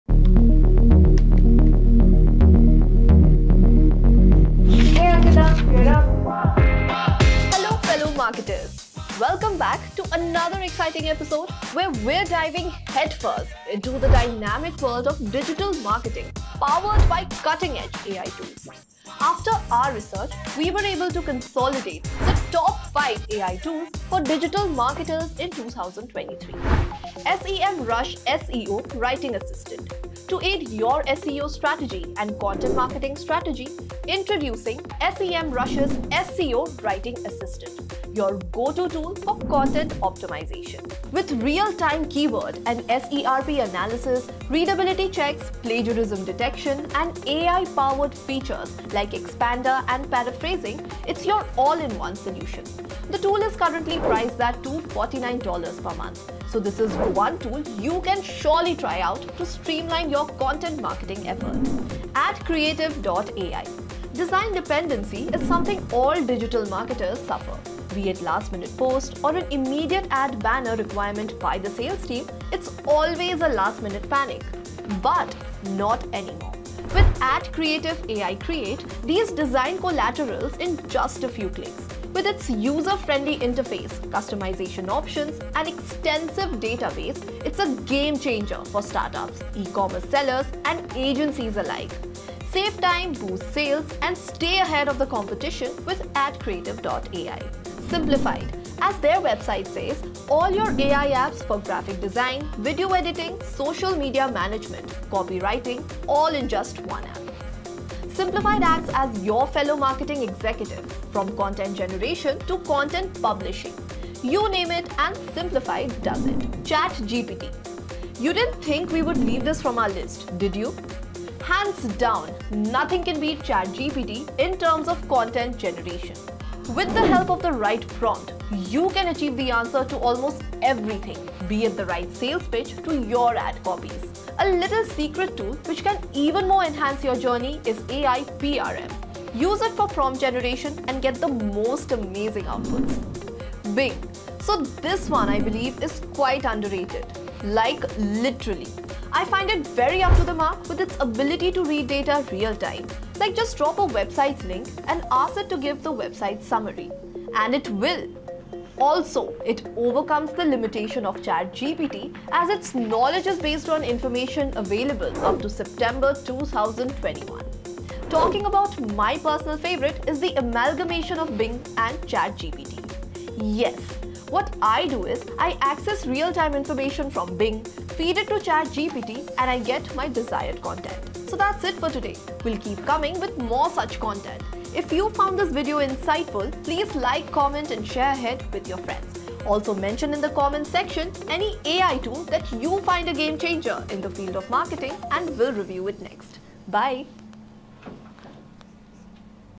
Top 5 AI tools for digital marketers
Welcome back, fellow marketers!
In this episode, we explore the exciting world of AI-powered digital marketing tools. Join us as we unveil the Top 5 AI tools for digital marketers in 2023, each designed to supercharge your marketing efforts.

Chapter 1: Semrush's SEO Writing Assistant
Discover Semrush's SEO Writing Assistant - your content optimization powerhouse. Real-time keyword analysis, readability checks, and more for $249/month. Stick around to find out how this tool can transform your content game.

Chapter 2: AdCreative.ai
Say goodbye to last-minute design panic with AdCreative.ai. Create design collaterals in a few clicks. Ideal for startups and agencies.

Chapter 3: Simplified
Simplified is your all-in-one AI app for graphic design, video editing, social media, and copywriting. A marketing Swiss Army knife. Don't miss the part where we showcase how Simplified acts as your marketing executive.

Chapter 4: Chat GPT
Unlock Chat GPT's content generation prowess. Craft sales pitches, ad copies, and more with the right prompts. Combine it with AIPRM for even better results. Stick around for our secret tips on maximizing Chat GPT's potential.

Chapter 5: Bing
Don't underestimate Bing! Real-time data reading and website summaries. Knowledge up to September 2021. Find out why Bing is a hidden gem and how it complements Chat GPT.

Chapter 6: Bonus - Bing + Chat GPT
Our personal favorite combo: Bing + Chat GPT. Access real-time data, feed it to Chat GPT, and watch your content creation soar. This fusion is a game-changer you won't want to miss.

That's a wrap for today's episode! But hold on, there's more. To get the full scoop on these AI tools and how they can revolutionize your marketing strategies, make sure to watch the entire video. Tell us your game-changing AI tools in the comments, and we might review them next.